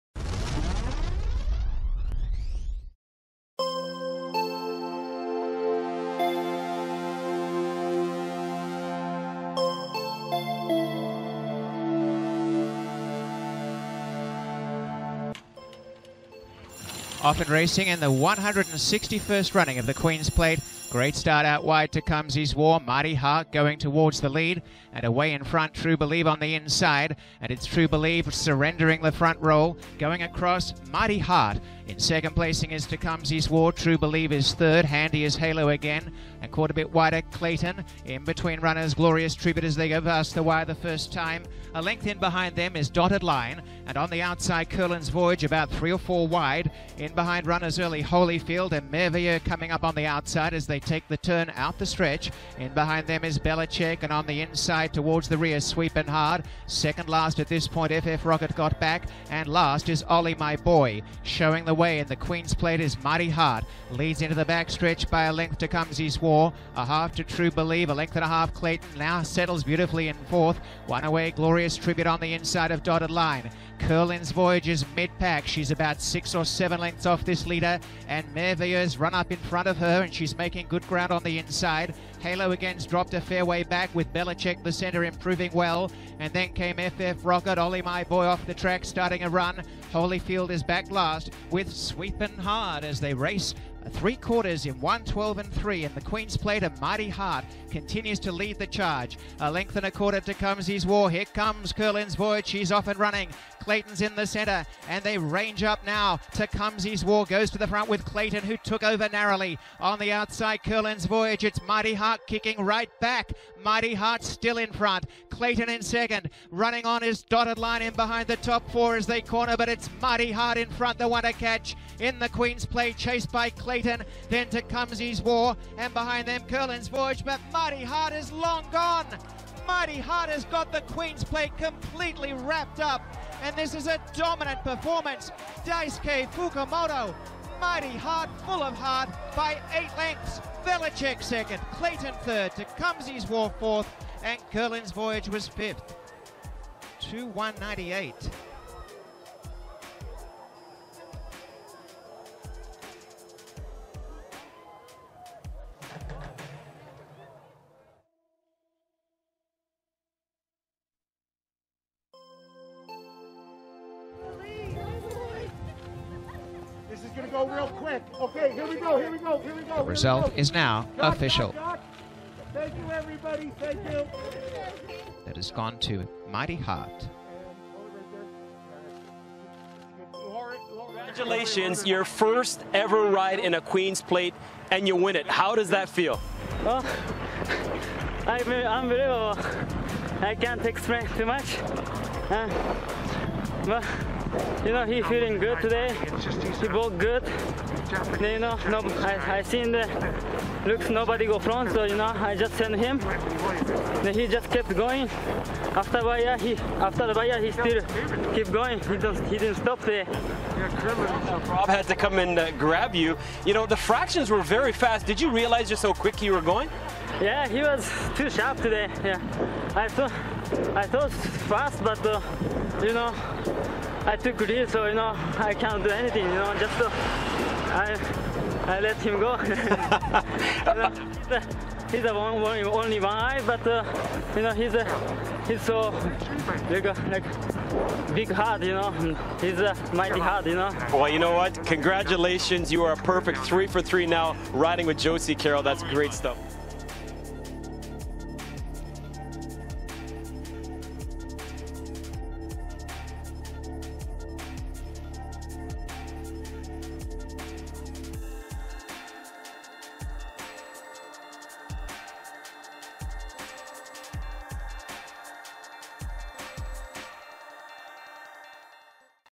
Queen's Plate 2020 Woodbine
161st Queen's Plate at Woodbine Racetrack

A brilliant showing from Mighty Heart wows viewers from all around the world.   You must watch to believe how much he wins by.  An outstanding horse that shows its talent on the big stage.  Will he win the Triple Crown?

Final Results
1st  Mighty Heart
2nd  Belichick
3rd  Clayton
4th  Tecumseh's War
5th  Curlin's Voyage
6th  Merveilleux
7th  Dotted Line
8th  Glorious Tribute
9th  Truebelieve
10th  Holyfield
11th  Olliemyboy
12th  F F Rocket
13th  Sweepin Hard
14th Halo Again